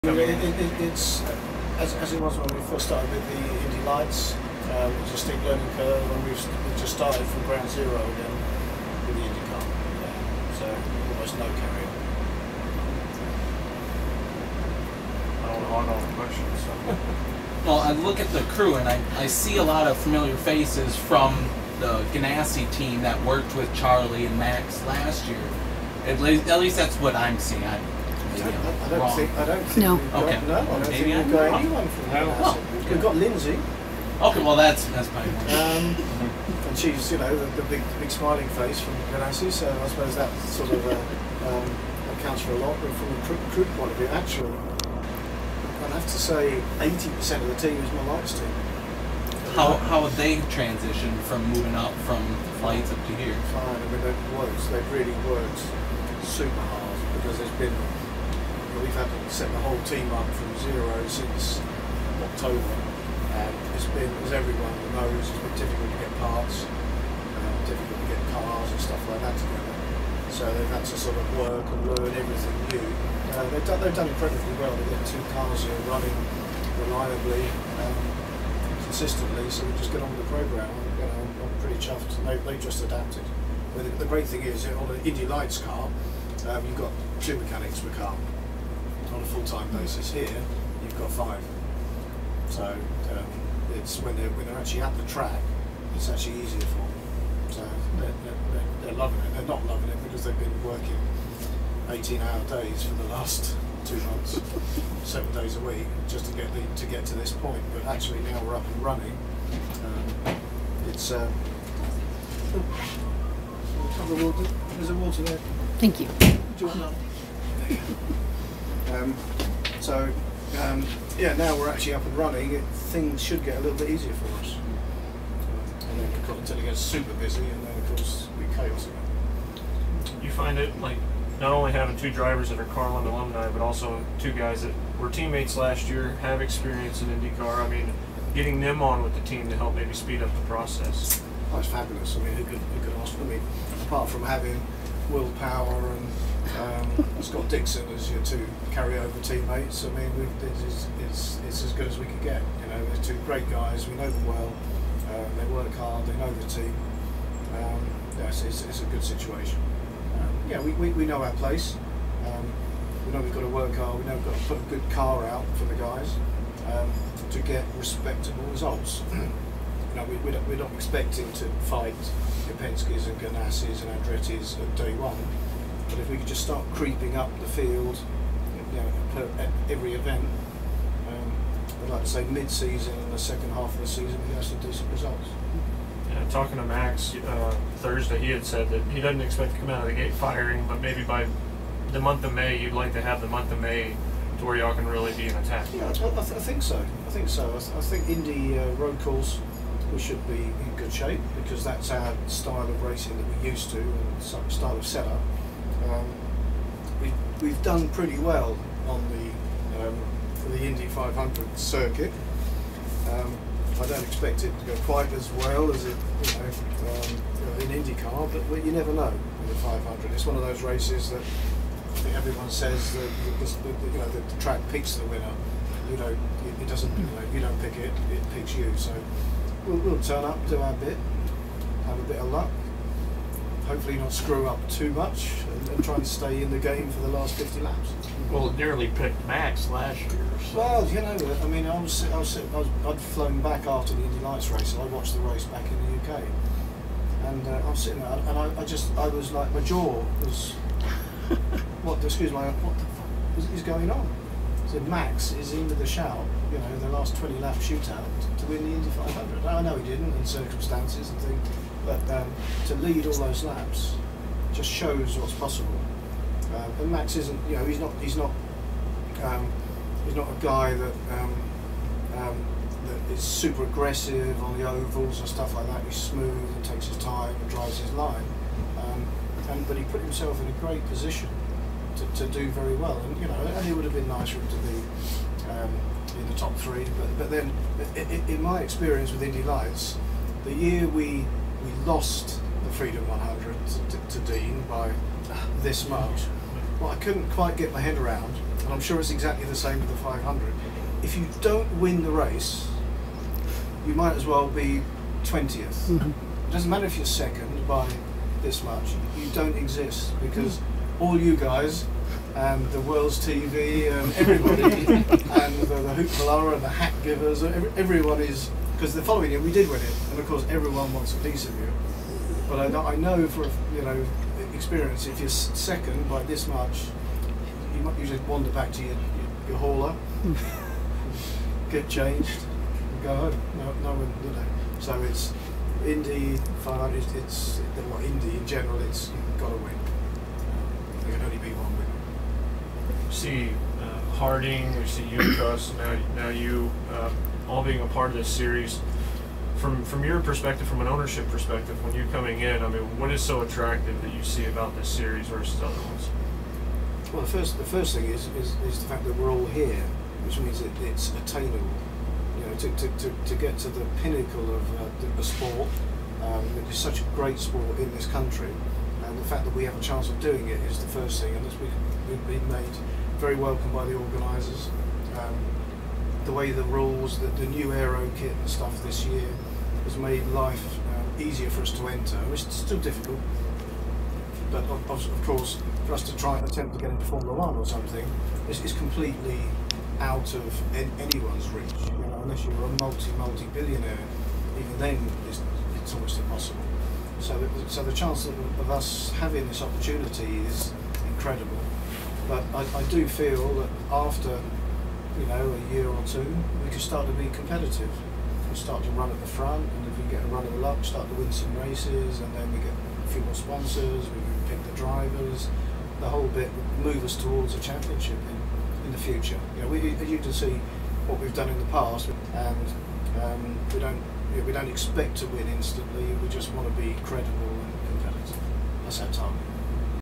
Q&A with Trevor Carlin in Phoenix
New IndyCar team owner Trevor Carlin held an impromptu Q&A session in his team trailer at ISM in Phoenix where the IndyCar teams were holding a preseason open test